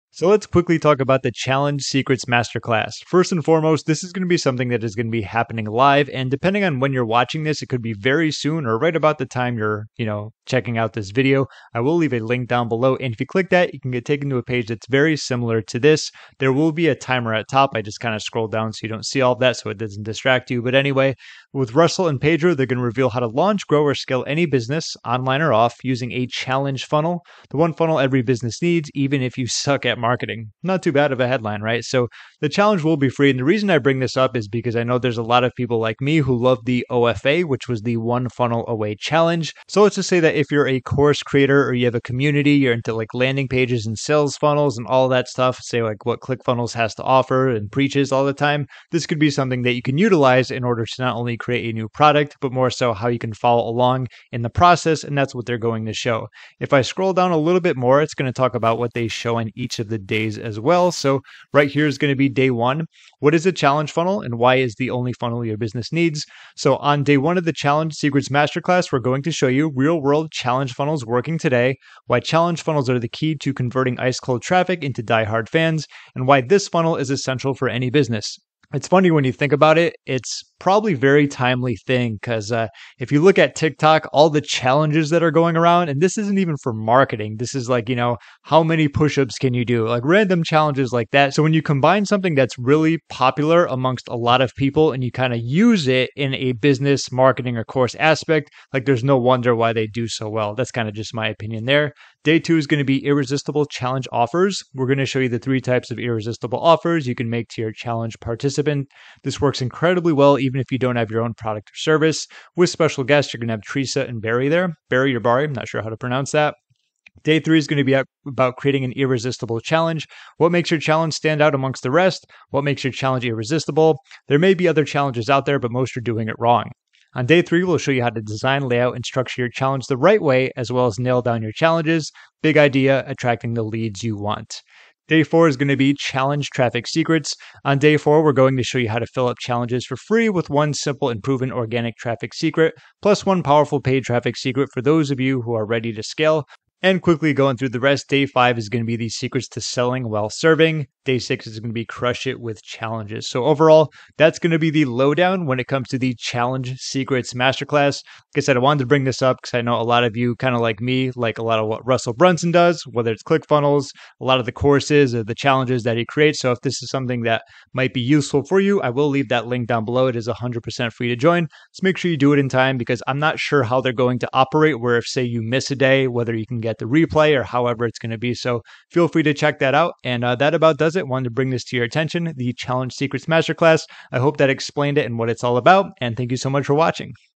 Challenge Secrets Masterclass Explained (What It Is & Overview)
Challenge Secrets Masterclass Explained Video

Thanks for checking out my Challenge Secrets Masterclass overview video. Here I will be talking about Russell's new live training that will be covering how to create challenges with your online or offline business. It is a live event that is free to join, and can have a lot of benefits if pulled off right. Keep in mind, that this isn't a Challenge Secrets Masterclass review yet, since it hasn't opened, but I might do that in the future. Anyway, these are the quick details so be sure to sign up for this to check out the free training.